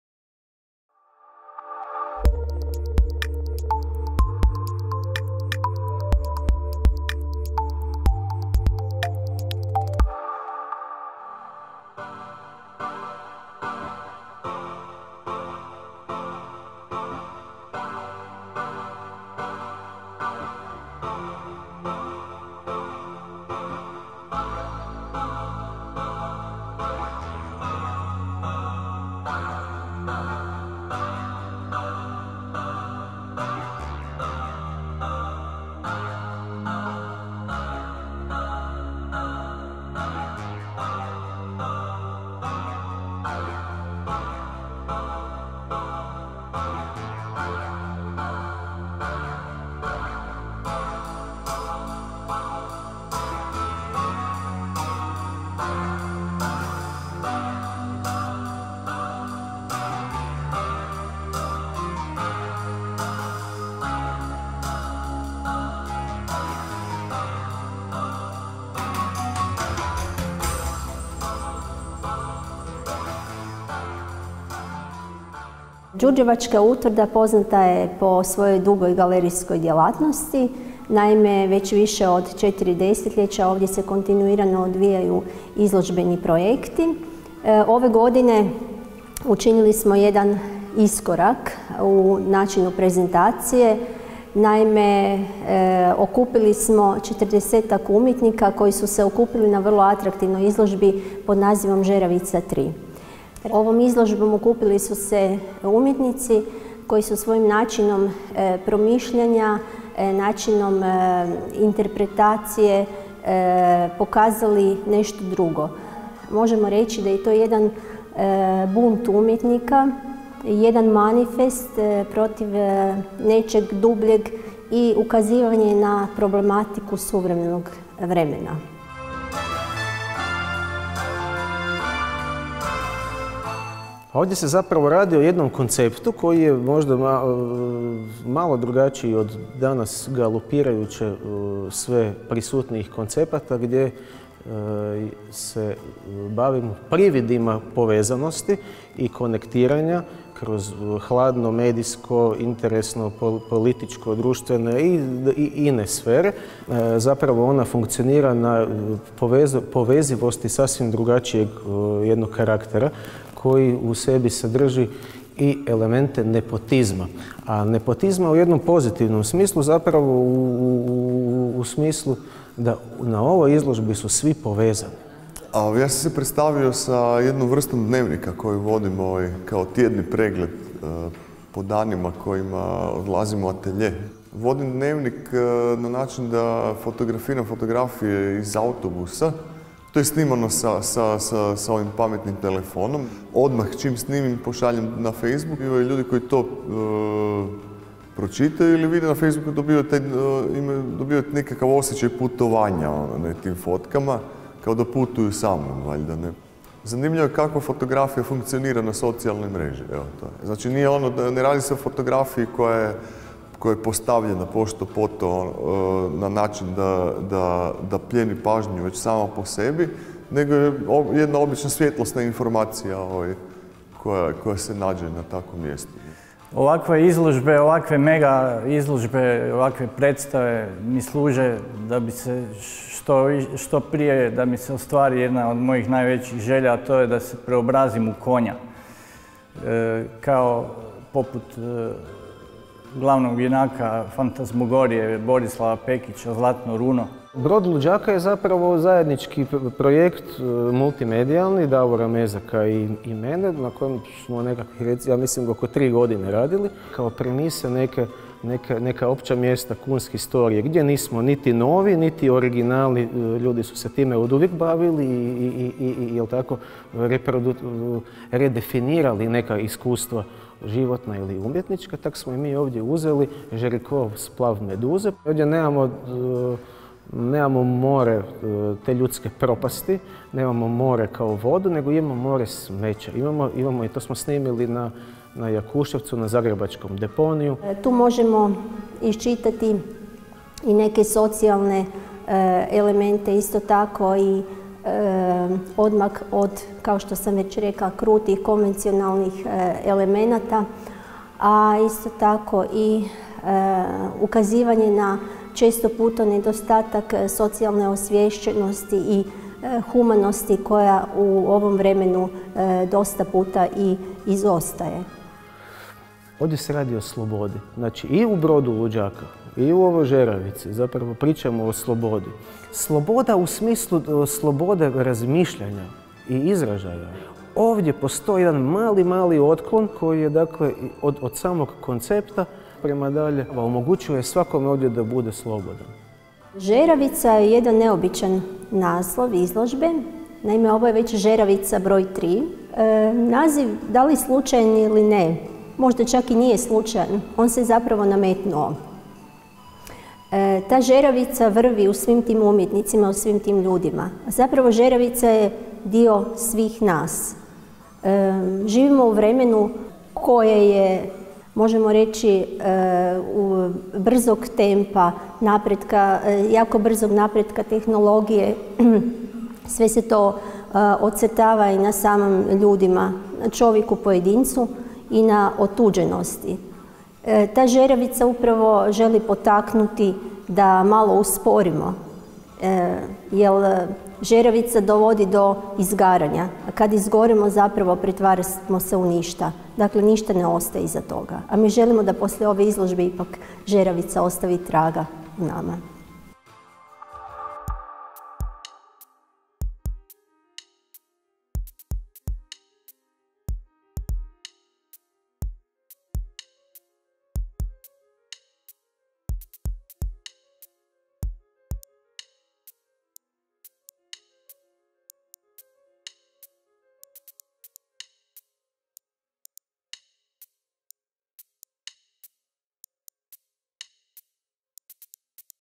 ŽERAVICA 3: THE MYSTYC (3ple Tunnel)
The multimedia exhibition Zeravica 3: The Mystyc (3ple tunnel) which is comprised of works created in traditional media, such as paintings, drawings and photographs, through installations, videos and performances, web art, etc., represents the continuation of multiannual art project that took place at the Museum of Djurdjevac city in 2015. The basic idea behind the project is a kind of an independence and resistance to generally imposed forms and rules of functioning in the art world which often hinder creativity. Bringing distinguished Croatian artists together with their guests from abroad represents a platform for contemplation about the relationship between content and form, with an emphasis on the positive inter-personal communication.

Author: Mirela Hunček
Shooting and video editing: Igor Paulić